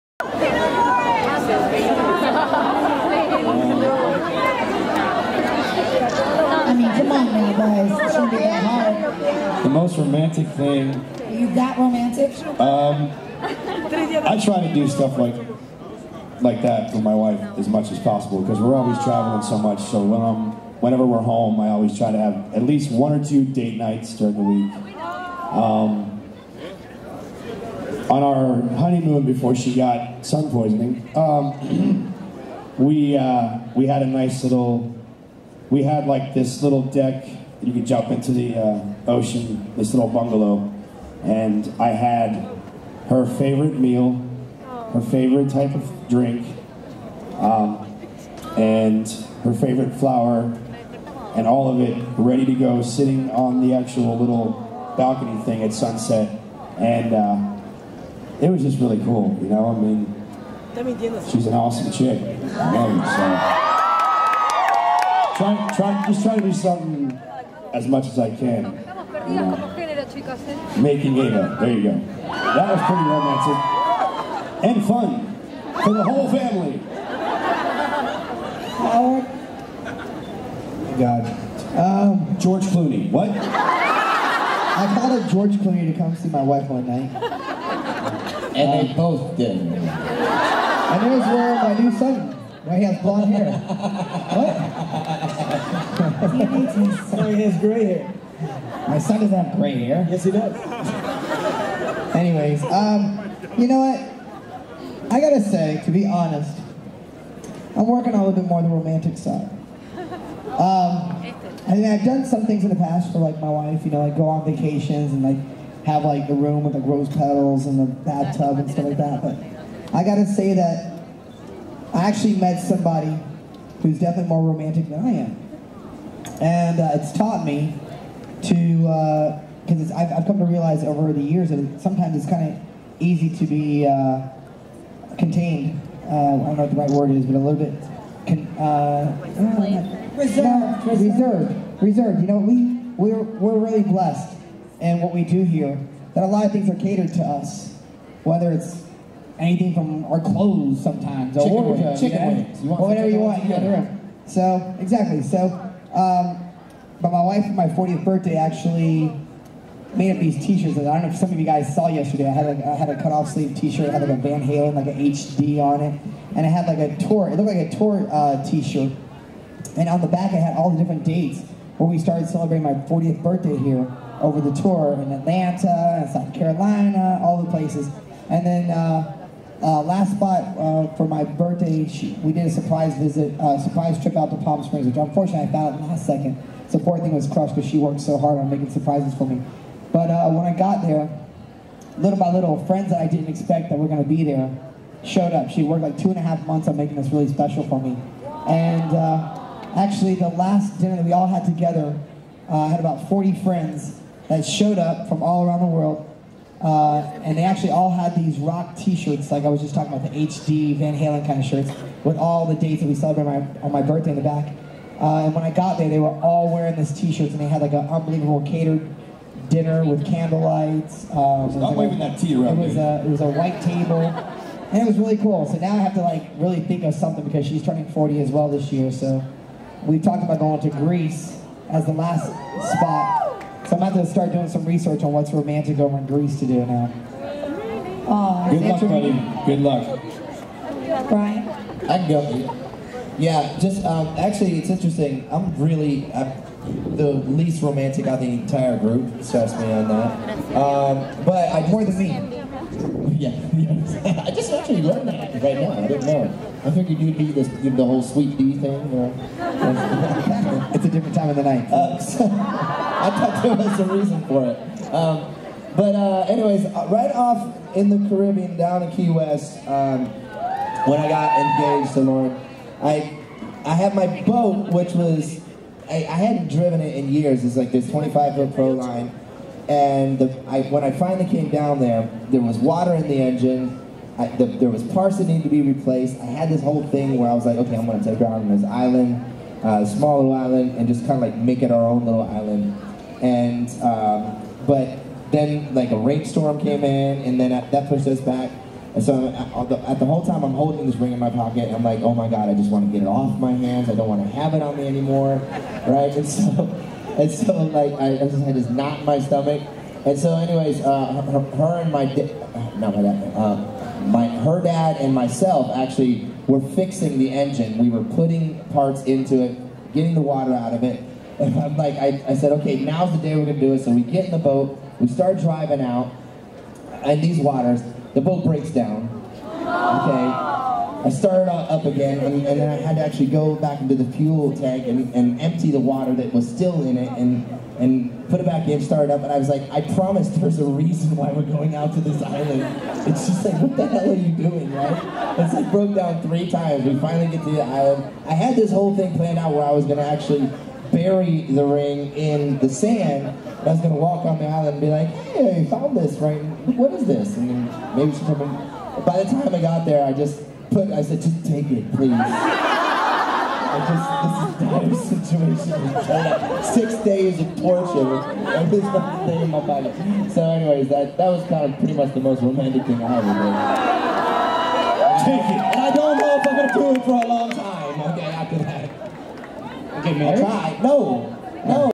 BSB Cruise 2013- Q&A: Most Romantic Gesture
Answering their most romantic gesture
Check out my blog for more info on Cruising with the Backstreet Boys bsbcruise.blogspot.com